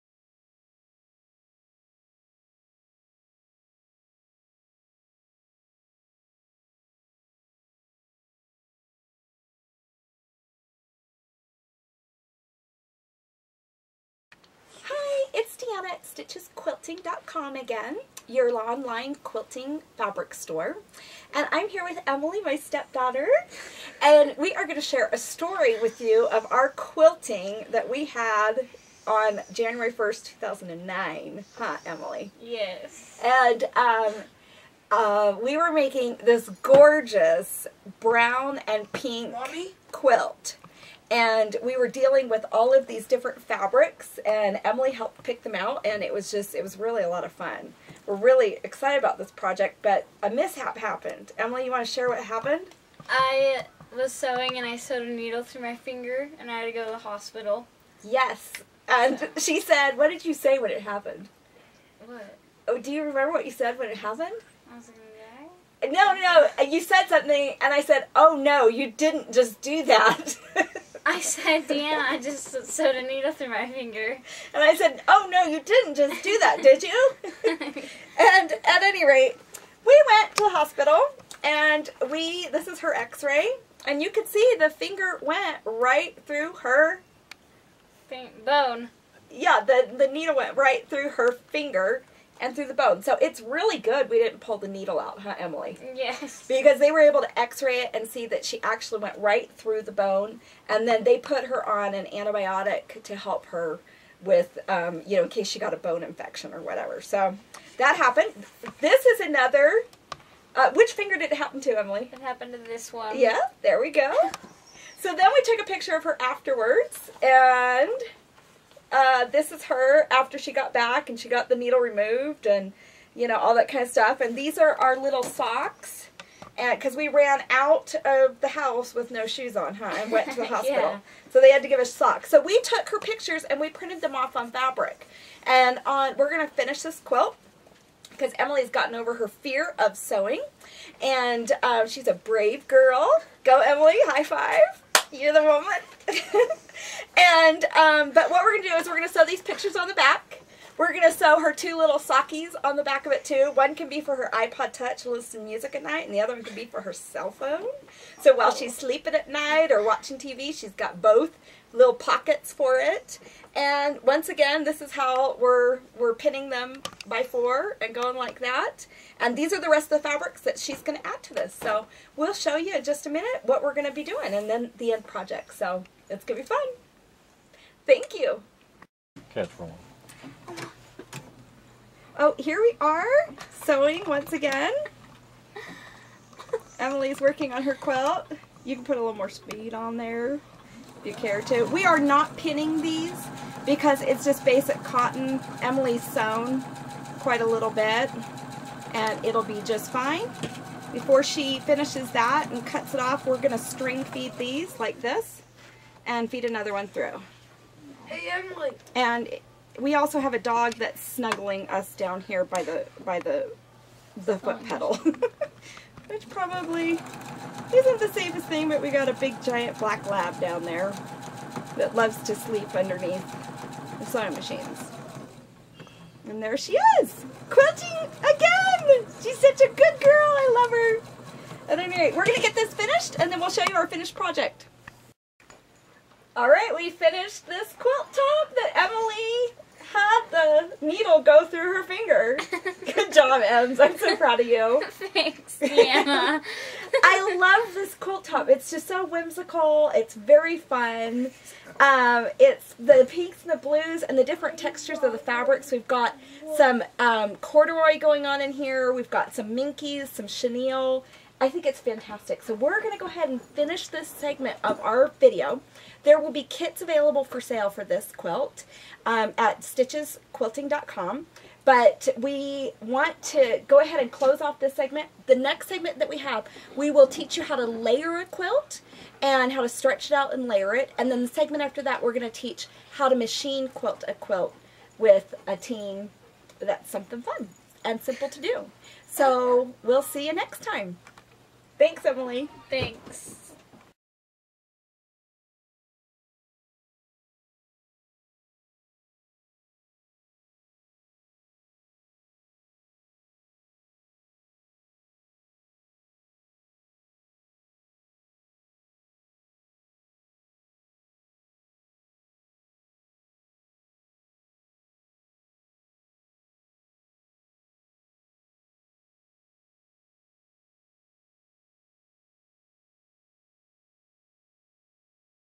How to Quilt with Your Teenager - Sewing Machine Needle went through the finer! Ouch!
Deanna at Stitches Quilting is quilting with her teenage daughter, Emily Wall.  Emily had sewn through her finger and was now brave enough to start quilting again.  Their story and the finished quilt top.  How to Quilt with Your Teenager - Sewing Machine Needle went through the finer! Ouch!  Excellent simple project to complete with any teenager that comes together quickly and with lots of pizazz.